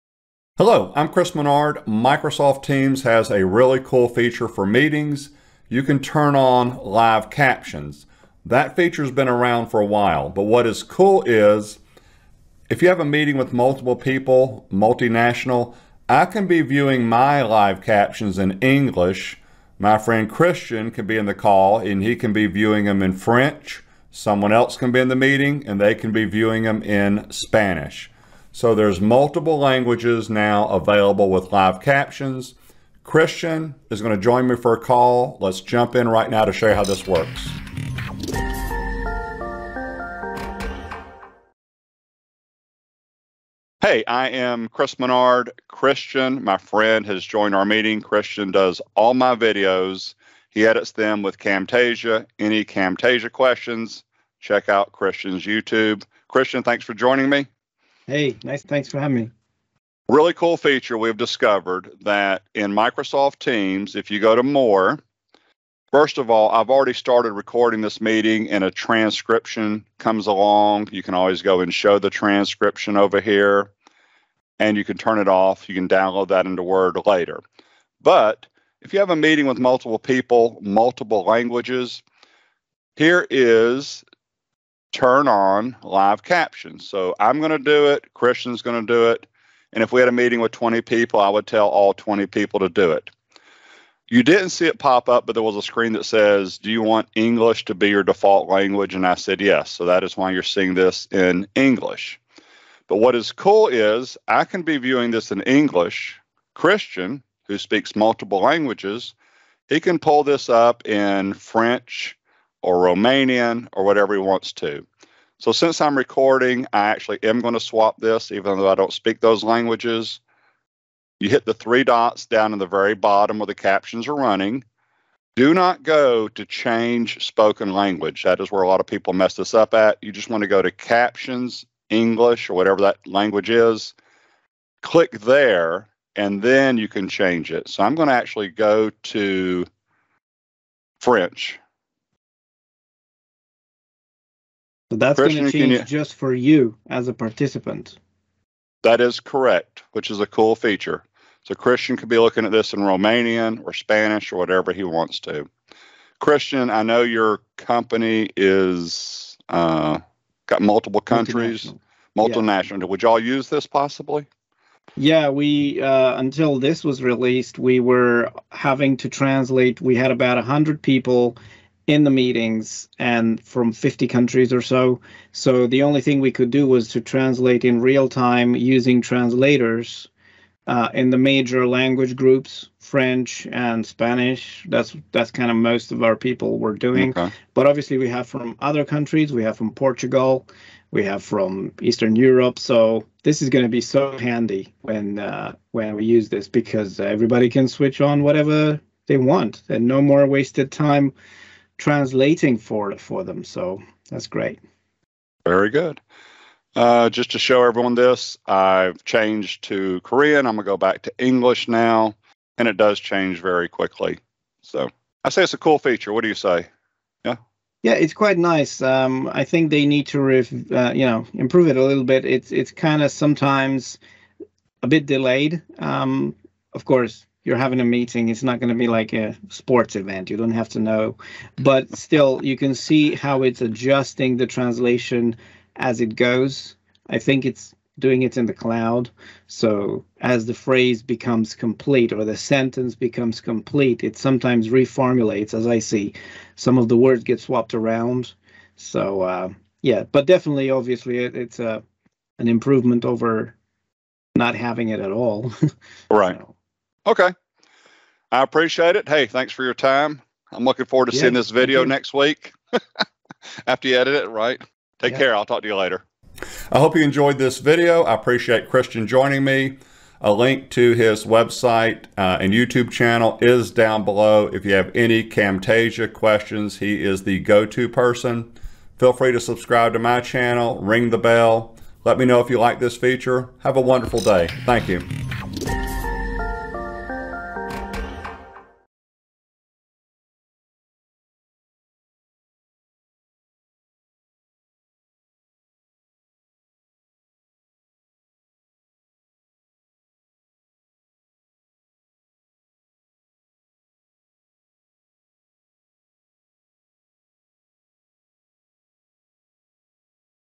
Teams Meetings - Captions in your preferred language | Multiple-Language Meetings
Teams Meetings allows live translated captions in the language you are most comfortable with. By default, live captions are displayed in the language that’s spoken during the meeting, but you can change the language of the captions. For example, ten people can be in English, four people in Spanish, and three in French. This is a great feature if you are having a multiple-language meeting.

Chapters:
0:00 Intro
0:37 Cristian joins my meeting
1:26 Turn on live captions
2:00 Change captions
4:28 Closing remarks

To use live translated captions:
Turn on live captions in your meeting.
Next to the meeting captions, select the More options button and click Subtitles.
Choose the language you want your captions translated to.

Supported languages - As of 10/19/22

-- EQUIPMENT USED
○ My camera